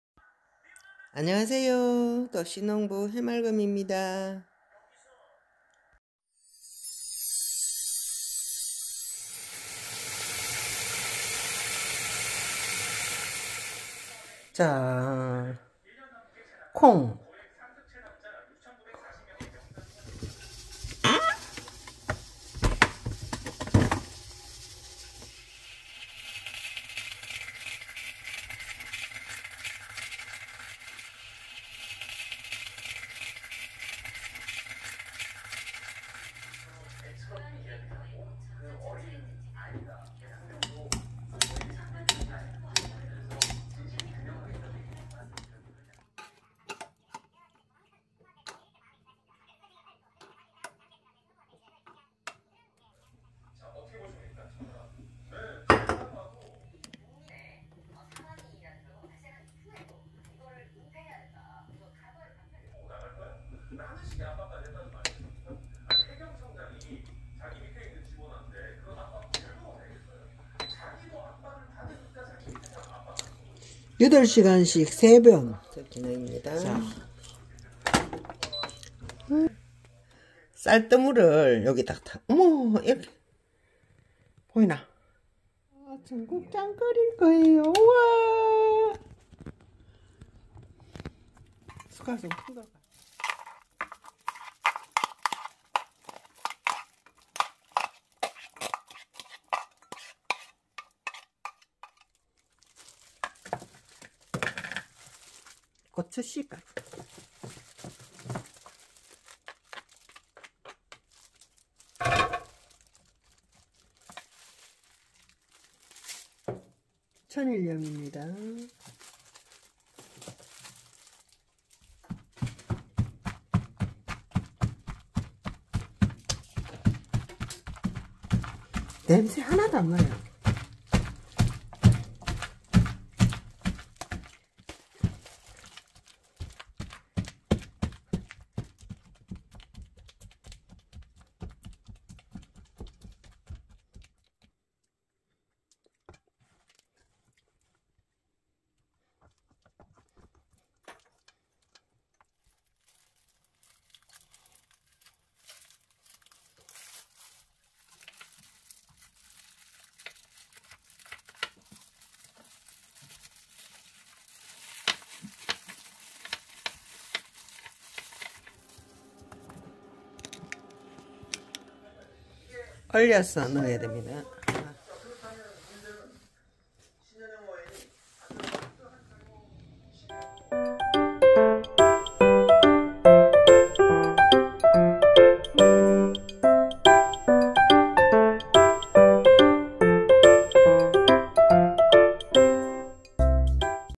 제빵기로 청국장 만들기 쌀뜨물 이용 쉬워요 보관법
수제 청국장